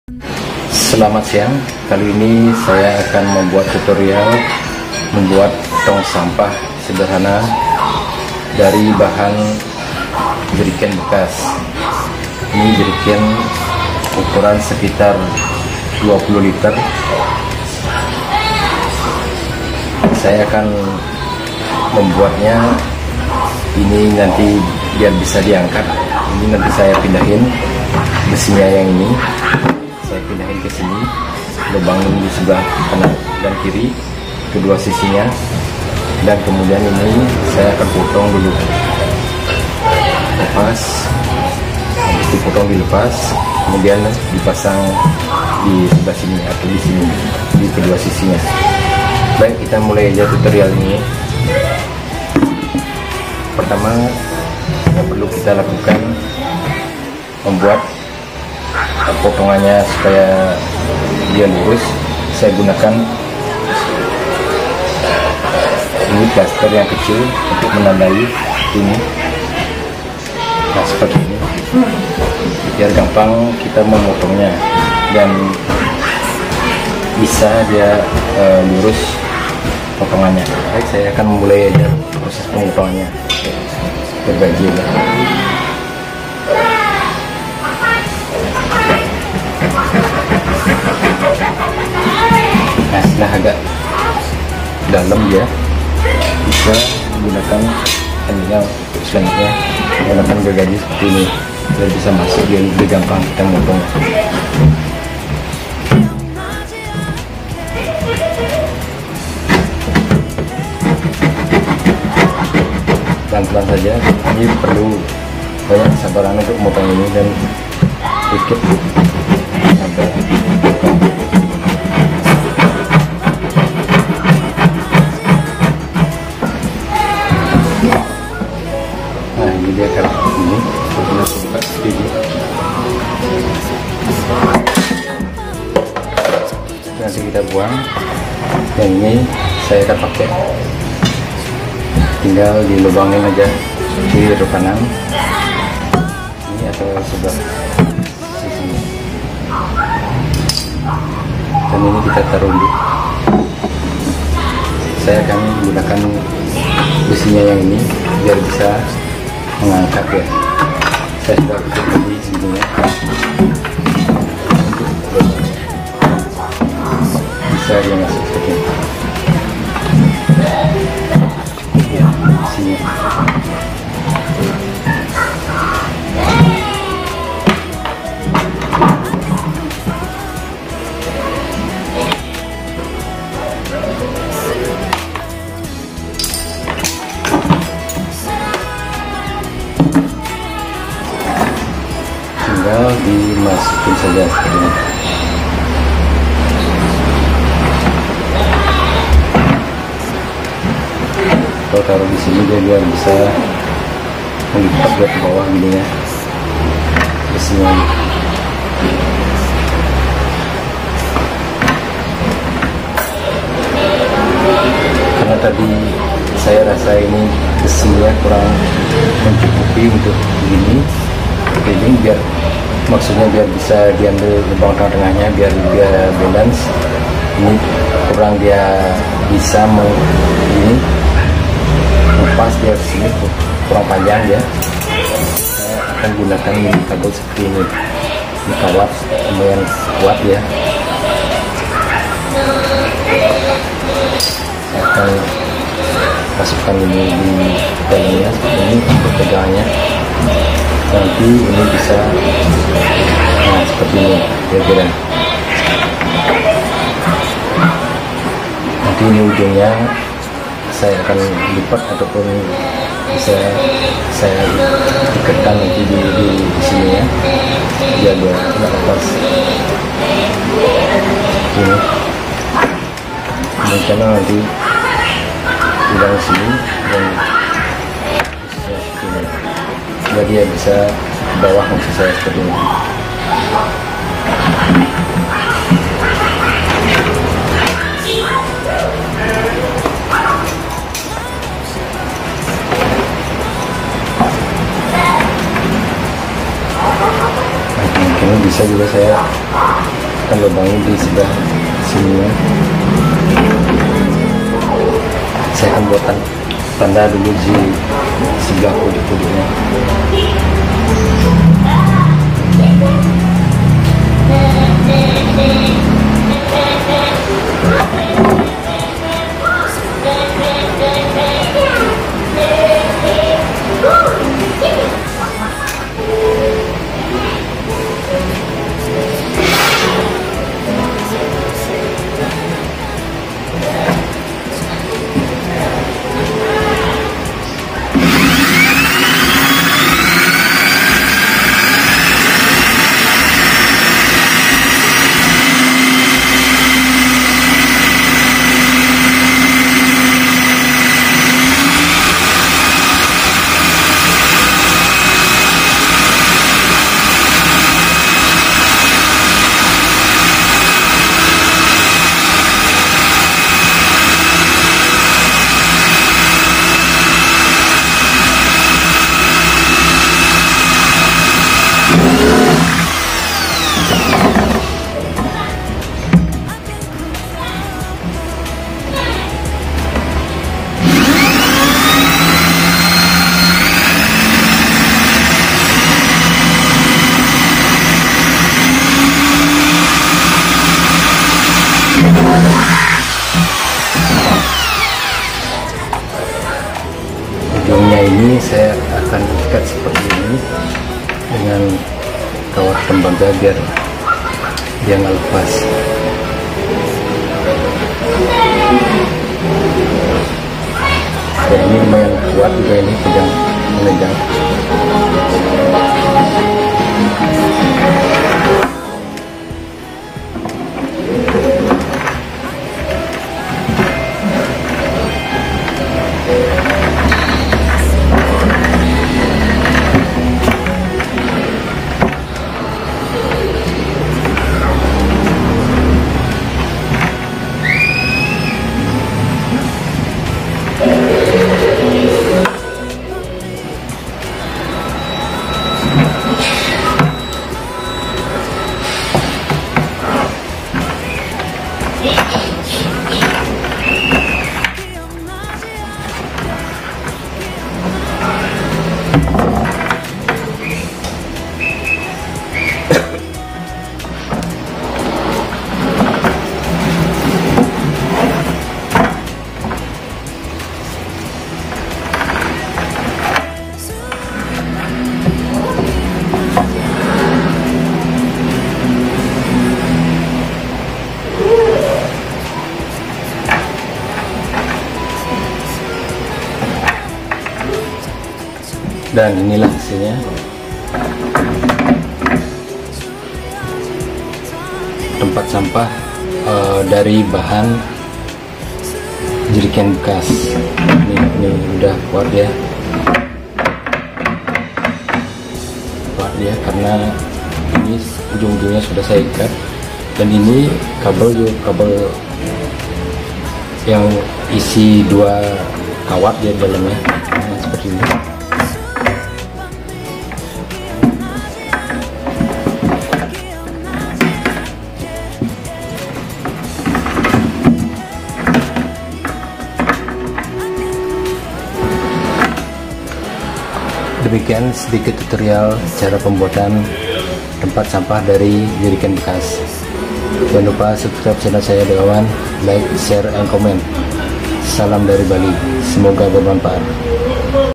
Tempat Sampah Dari Jerigen Bekas
Cara Membuat Tempat Sampah Sederhana Dari
jerigen Bekas.

Semoga Bermanfaat 👍